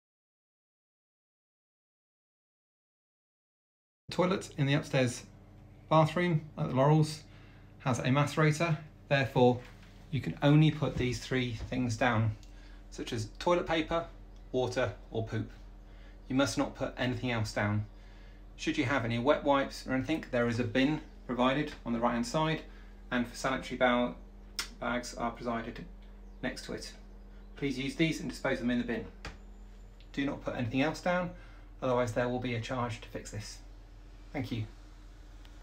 The Laurels - Guidance regarding the upstairs macerator toilet
This video explains how to appropriately use the upstairs macerator toilet at The Laurels. There are only three things that should go down the toilet; poop, water and toilet paper. Nothing else is to be put down this otherwise it will get damaged. Thank you for your co-operation.

Follow us for the latest information and news;